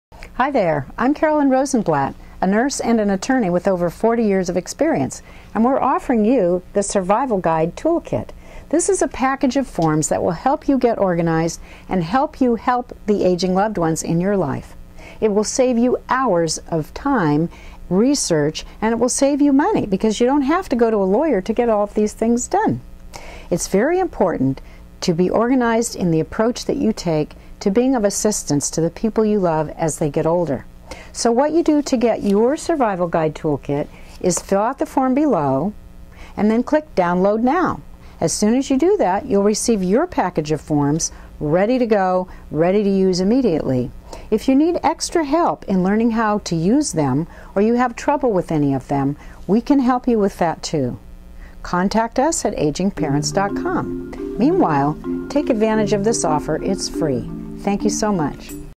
The Survival Guide Toolkit From AgingParents.com
The Survival guide toolkit includes critical legal and healthcare forms like power of attorney, or healthcare directive, or POLST, and many checklists. The toolkit helps your organize important information your loved ones will need to protect you wishes.